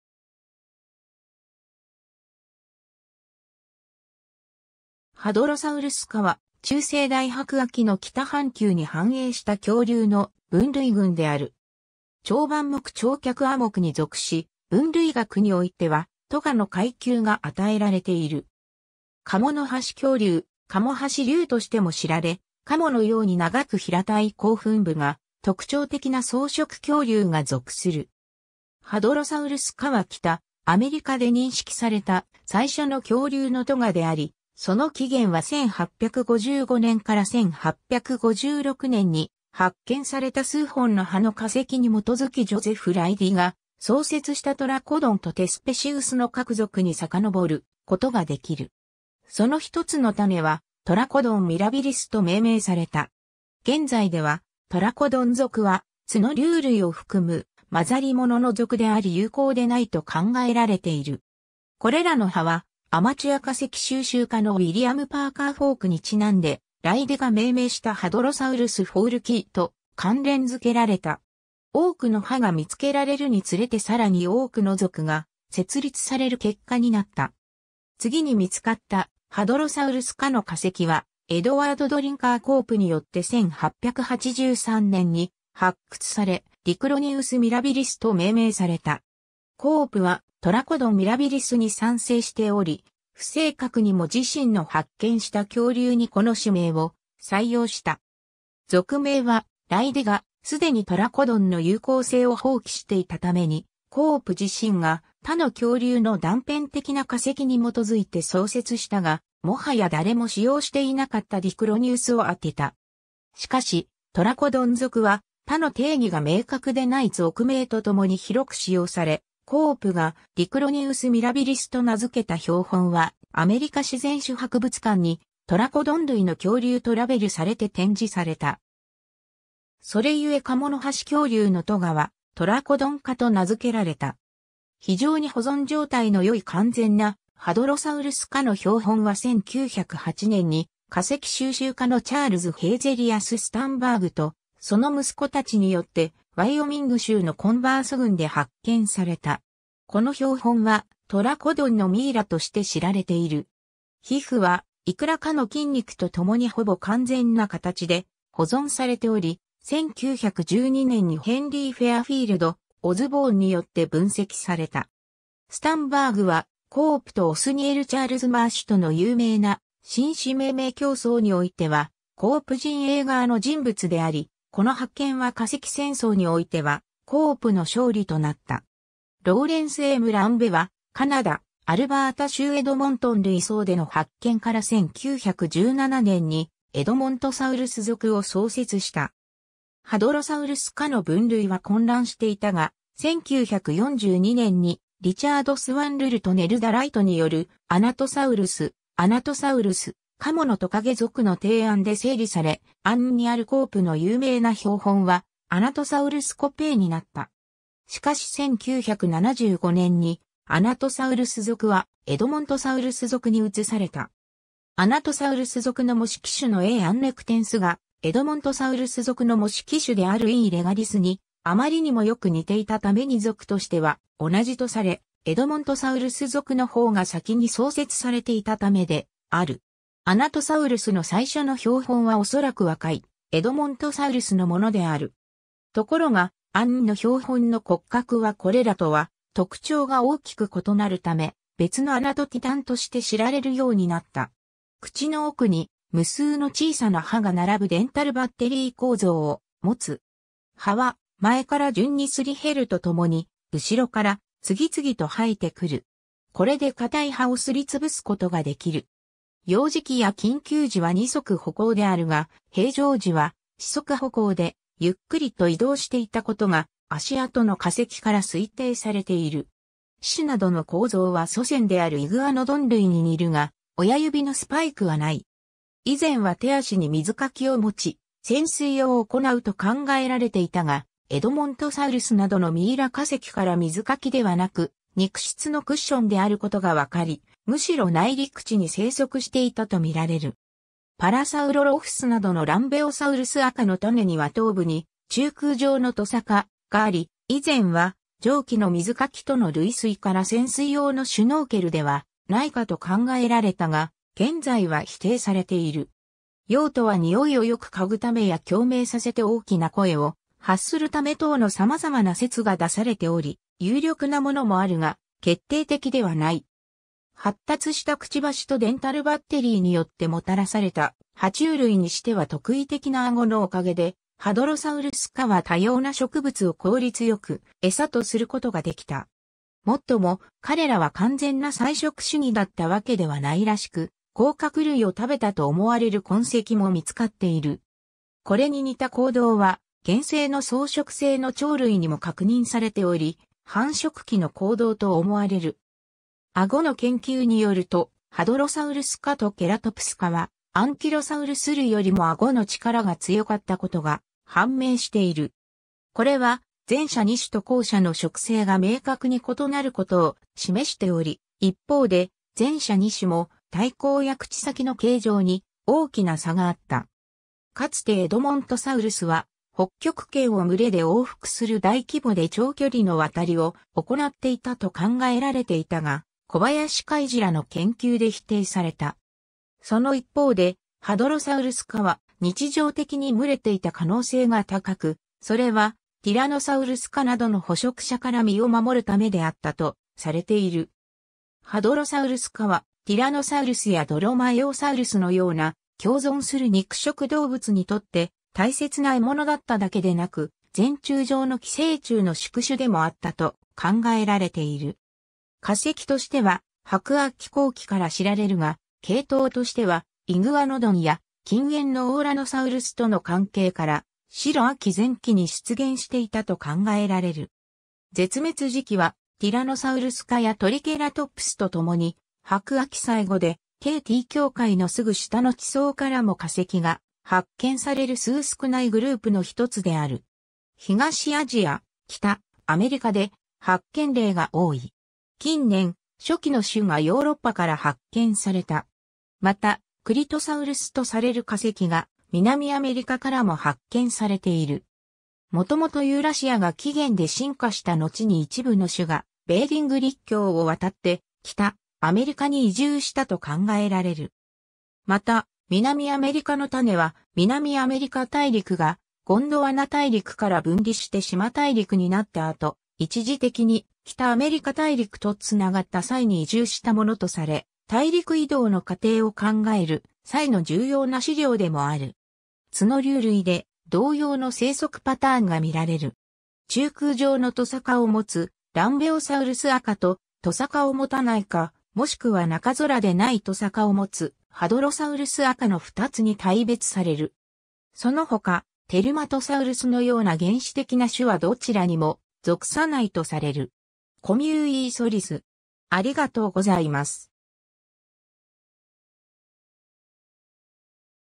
ハドロサウルス科
ハドロサウルス科 (Hadrosauridae) は中生代白亜紀の北半球に繁栄した恐竜の分類群である。
鳥盤目 - 鳥脚亜目に属し、分類学においては科の階級が与えられている。
カモノハシ恐竜、カモハシ竜としても知られ、鴨のように長く平たい口吻部が特徴的な草食恐竜が属する。
 ハドロサウルス科は北アメリカで認識された最初の恐竜の科であり、その起源は1855年-1856年に発見された数本の歯の化石に基づきジョゼフ・ライディ (Joseph Leidy) が創設したトラコドン (Trachodon) とテスペシウス (Thespesius) の各属に遡ることができる（このとき他に獣脚類のトロオドン (Troodon) とデイノドン (Deinodon)、曲竜のパレオスキンクス (Plaeosucincus) 属も創設された）。
その一つの種はトラコドン・ミラビリス (Trachodon mirabilis) と命名された。
現在ではトラコドン属は角竜類を含む混ざり物の属であり有効でないと考えられている。
これらの歯はアマチュア化石収集家のウィリアム・パーカー・フォーク (Willium Parker Foulke) にちなんでライディが命名したハドロサウルス・フォウルキー (Hadrosaurus foulkii) と関連付けられた。
多くの歯が見つけられるにつれてさらに多くの（現在では廃れた）属が設立される結果になった。
 次に見つかったハドロサウルス科の化石はエドワード・ドリンカー・コープによって1883年に発掘され、ディクロニウス・ミラビリス (Diclonius mirabilis) と命名された。
コープはトラコドン・ミラビリスに賛成しており不正確にも自身の発見した恐竜にこの種名を採用した。
属名は、ライディが既にトラコドンの有効性を放棄していたためにコープ自身が他の恐竜の断片的な化石に基づいて創設したがもはや誰も使用していなかったディクロニウスをあてた。
しかし、トラコドン属は他の定義が明確でない属名と共に広く使用され、コープがディクロニウス・ミラビリスと名付けた標本はアメリカ自然史博物館 (AMNH) にトラコドン類の恐竜とラベルされて展示された。
それゆえカモノハシ恐竜の科はトラコドン科 (Trachodontidae) と名づけられた。
 非常に保存状態のよい完全なハドロサウルス科の標本（今日ではエドモントサウルス・アンネクテンスとされている）は1908年に化石収集家のチャールズ・ヘイゼリアス・スタンバーグ (Charles Hazelius Sternberg) とその息子たちによってワイオミング州のコンヴァース郡で発見された。
この標本は“トラコドンのミイラ”として知られている。
皮膚はいくらかの筋肉と共にほぼ完全な形で保存されており、1912年にヘンリー・フェアフィールド・オズボーン によって分析された。
スタンバーグはコープとオスニエル・チャールズ・マーシュとの有名な新種命名競争においてはコープ陣営側の人物であり、この発見は化石戦争 (Bone Wars) においてはコープの勝利となった。
 ローレンス・M・ランベ (Lawrence M Lambe) はカナダ、アルバータ州エドモントン累層での発見から1917年にエドモントサウルス属（エドモントンのトカゲ）を創設した。
 ハドロサウルス科の分類は混乱していたが、1942年にリチャード・スワン・ルル(Richard Swann Lull) とネルダ・ライト (Nelda Wrigh) によるアナトサウルス (Anatosaurus、鴨のトカゲ）属の提案で整理され、AMNHにあるコープの有名な標本はアナトサウルス・コペイになった。
しかし1975年にアナトサウルス属はエドモントサウルス属に移された。
アナトサウルス属の模式種のA.アンネクテンスがエドモントサウルス属の模式種であるE.レガリス (E.regalis) にあまりにもよく似ていたために属としては同じとされ、エドモントサウルス属の方が先に創設されていたためである。
アナトサウルスの最初の標本はおそらく若いエドモントサウルスのものである。
ところが、AMNHの標本の骨格はこれらとは特徴が大きく異なるため、別のアナトティタン (Anatotitan、巨大な鴨）属が創設され、1990年よりアナトティタン・コペイ (Anatotitan copei) として知られるようになった。
 口の奥に無数の小さな歯が並ぶ"デンタルバッテリ...